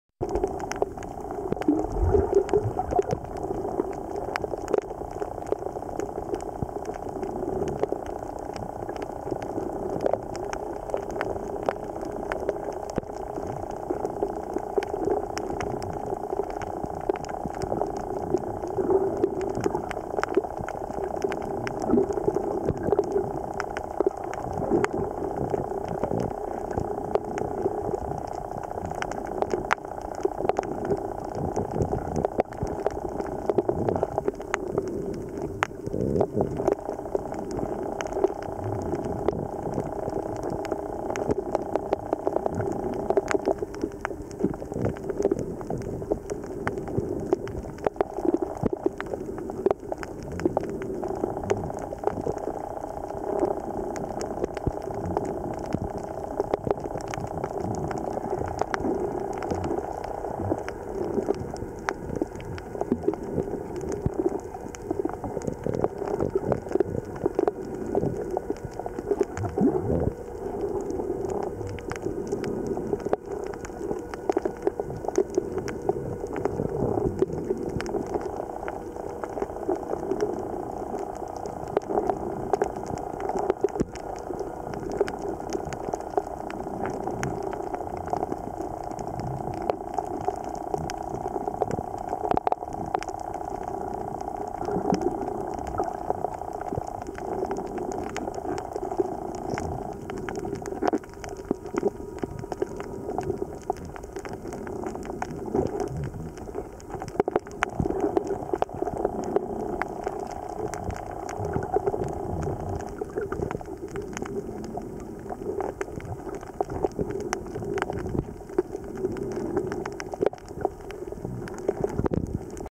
Convict Surgeon Fish Schooling
Convict surgeon fish schooling on the reef at Gab Gab.